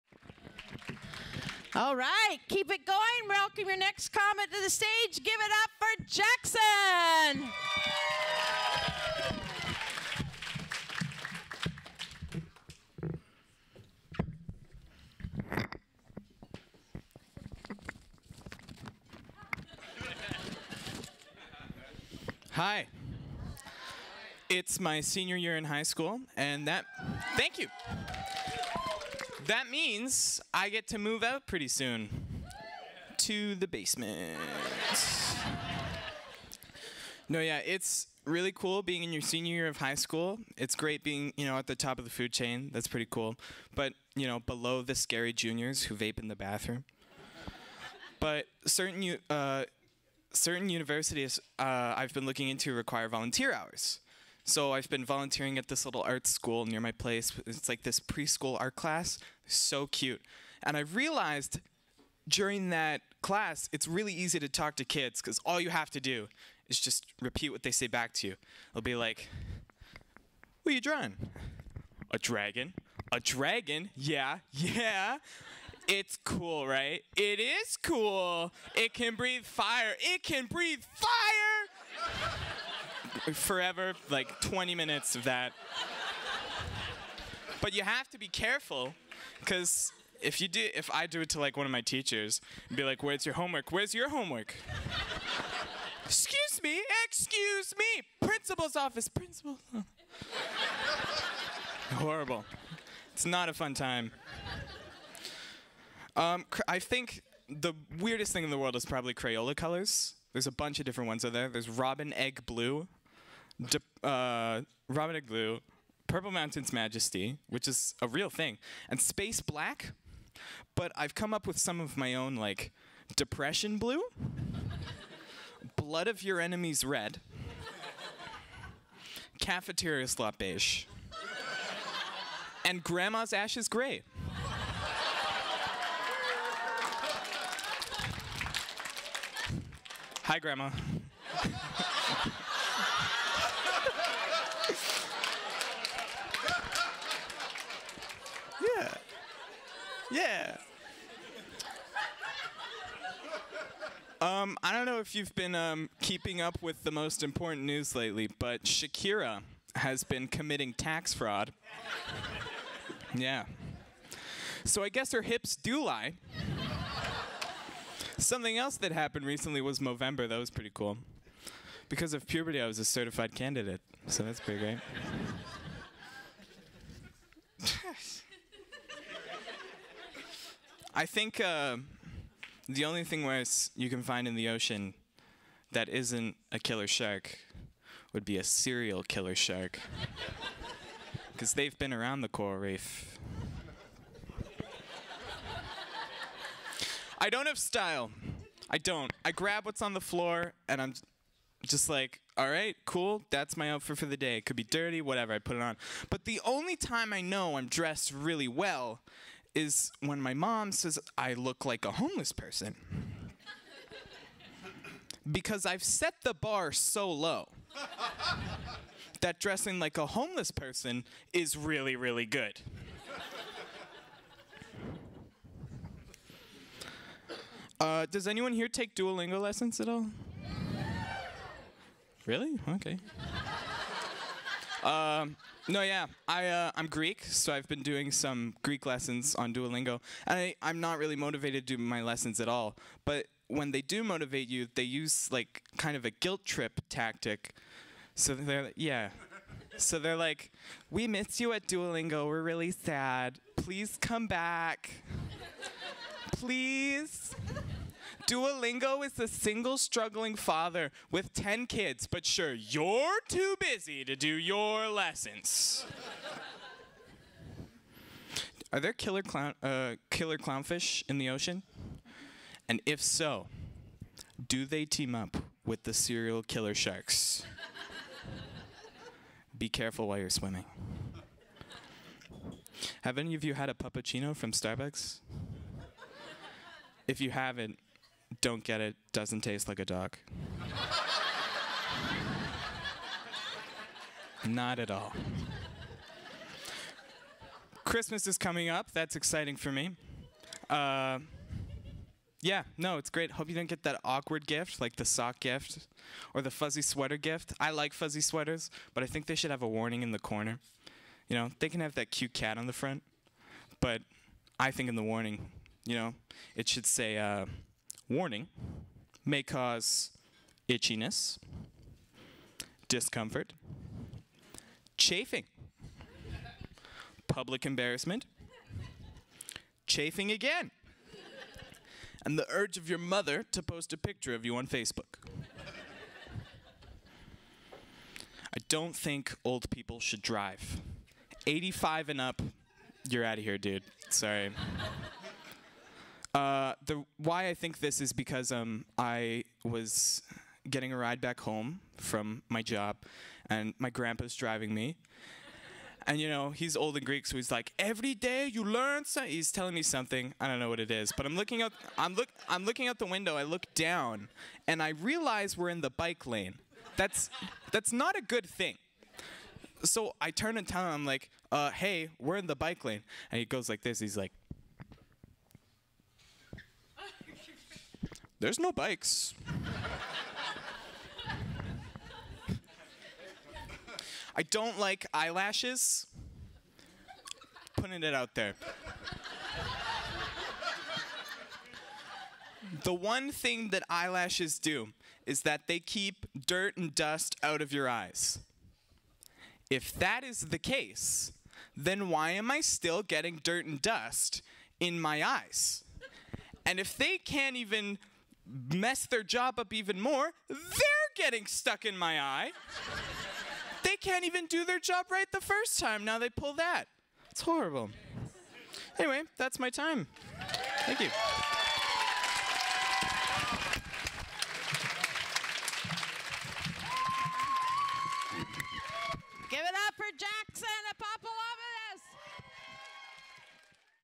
Jackson Apostolopoulos
LaughterZone 101 student performs at the LaughterZone 101 gala, Friday December 2nd, 2022

Have you been told you are funny? Want to try stand-up comedy for yourself? New stand-up comedy classes starting soon!